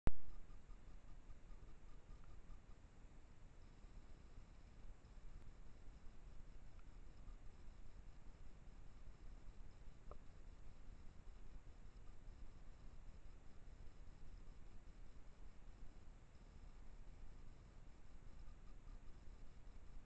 Nachtleben in Wesel 5, zwei Igel haben sich gefunden.
Aufgenommen mit einer Infrarotkamera in Wesel Flüren